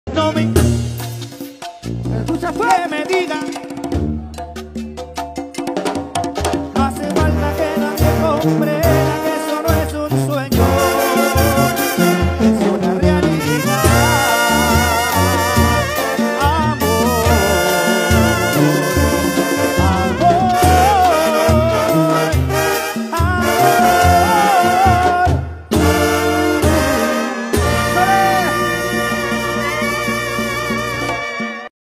‼️ Músico besa a fan en pleno concierto 😱
Durante un evento, Ramón Maldonado, vocalista de La Original Banda El Limón, se acercó a cantarle a una de sus fans y al final le dio un beso.

👩‍❤️‍💋‍👨 Seguramente ella jamás olvidará ese momento 💋